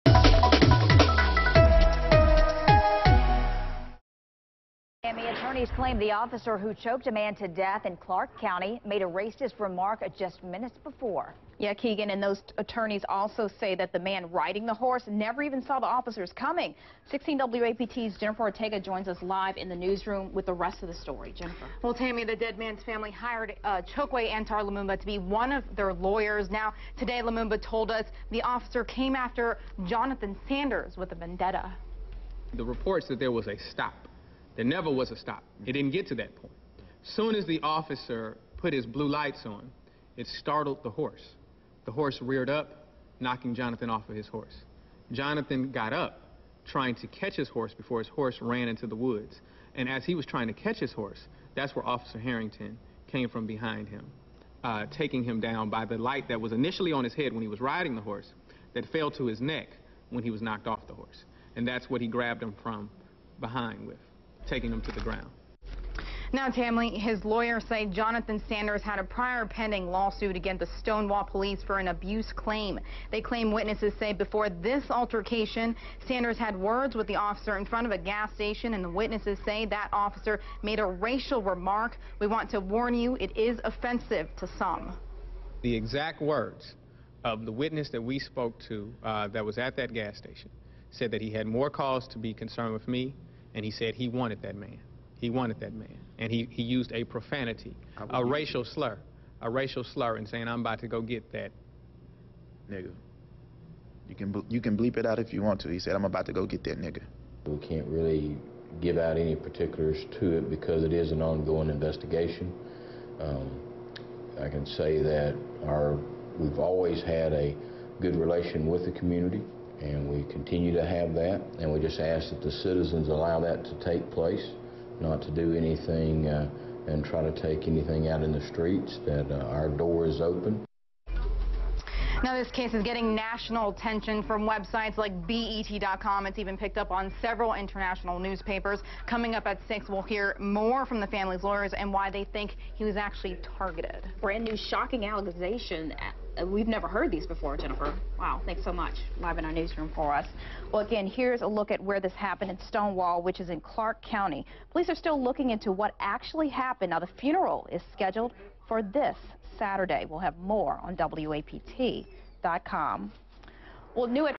Attorneys claim the Stonewall death was racist
Attorneys are claiming that a racial slur was said before the death Jonathan Sanders.